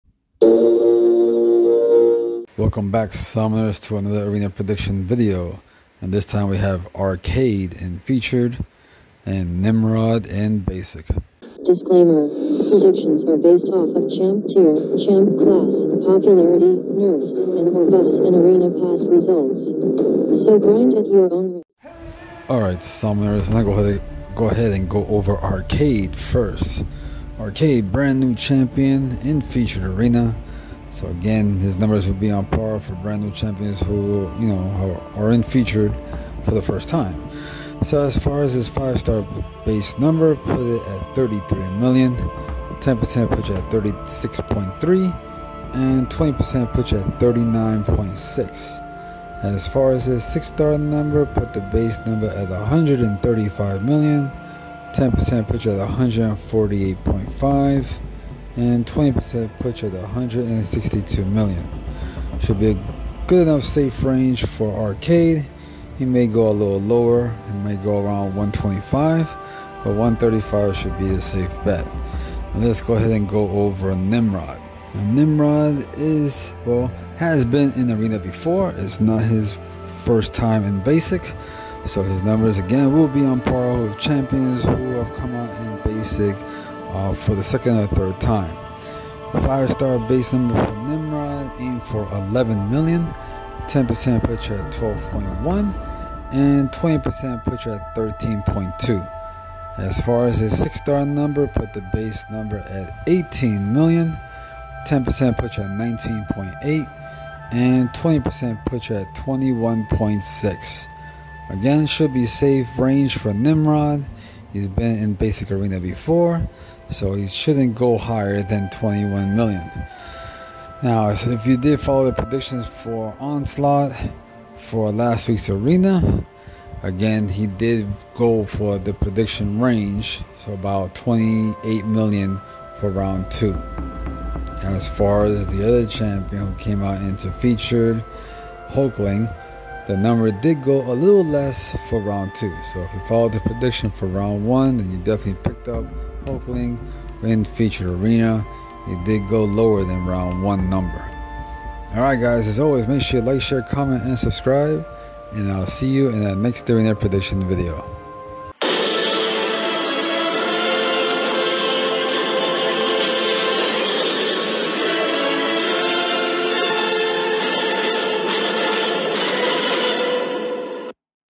MCOC Arcade arena predictions | MCOC Nimrod arena predictions | round 1
MCOC Arcade and Nimrod round 1 arena predictions.
I do a little data analysis by using past results of the basic and featured arena champs to come up with arena cutoff round 1 predictions for Arcade and Nimrod in Marvel Contest of Champions.